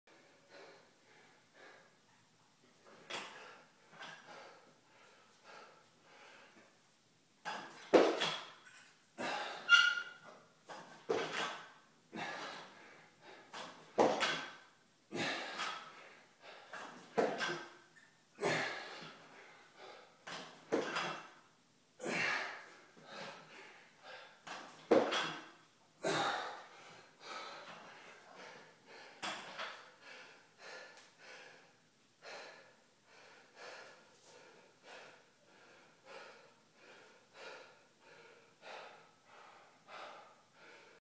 Squat snatch - CF 2266
Squat snatch with rep starting as weight just goes below knee. Part of 3x 30 wall balls, 30 snatch with 75#